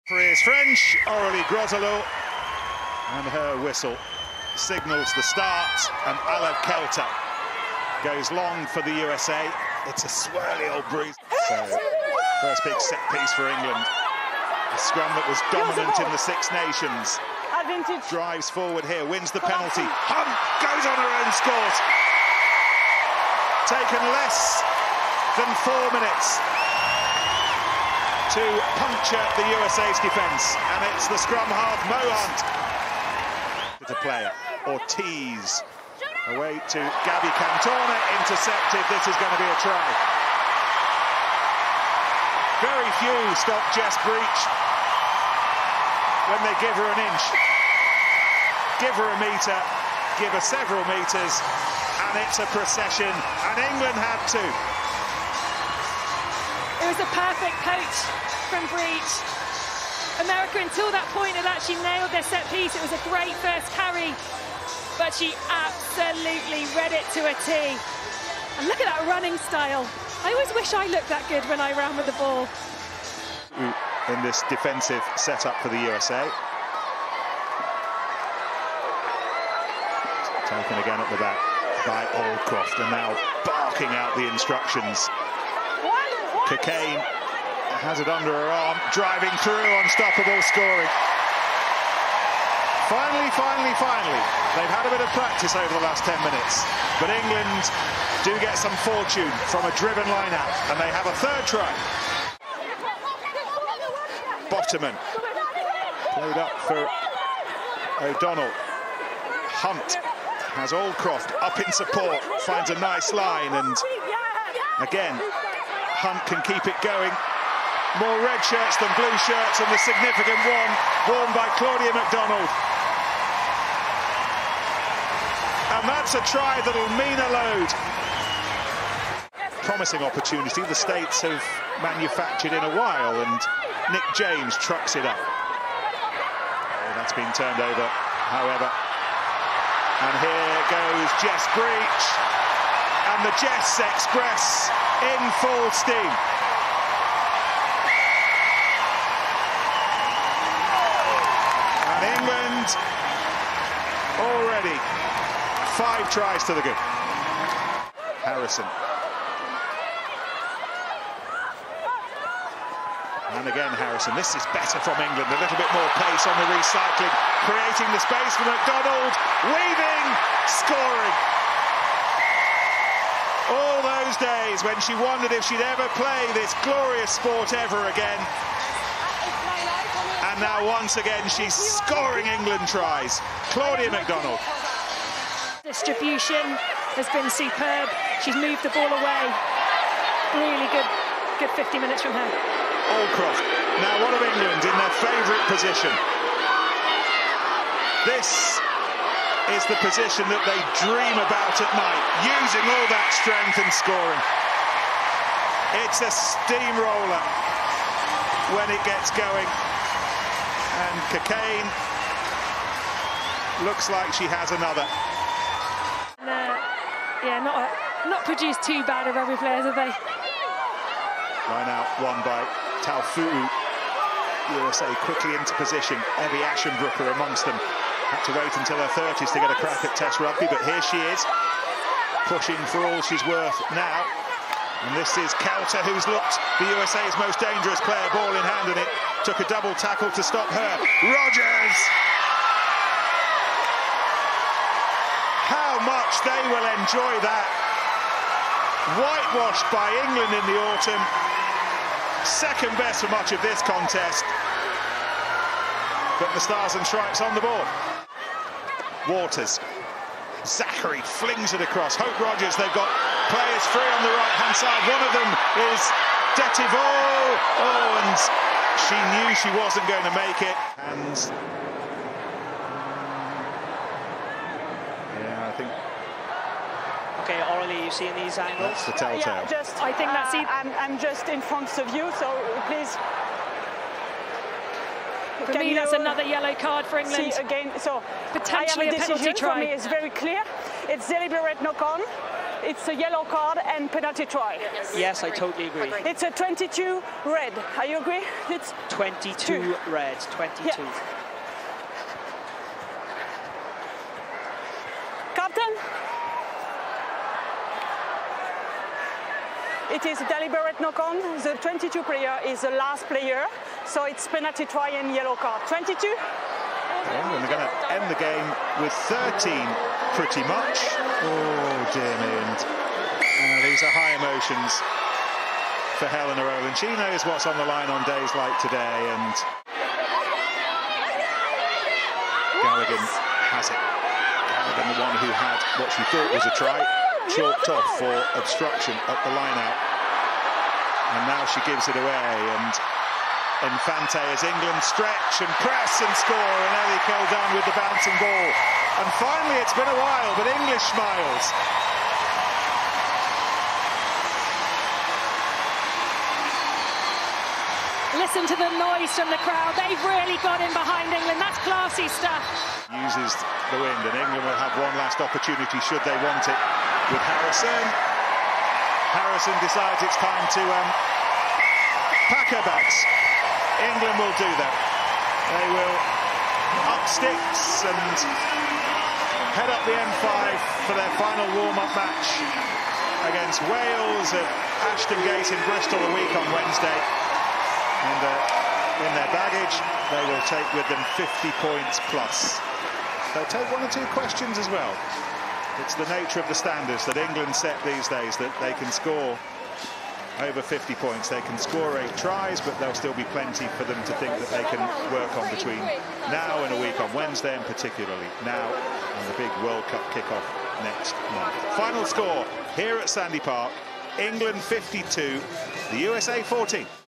📽 Highlights | Red Roses 52-14 USA | Eight tries in Exeter
The Red Roses run in eight tries at Sandy Park as they beat USA in the Summer Nations Series.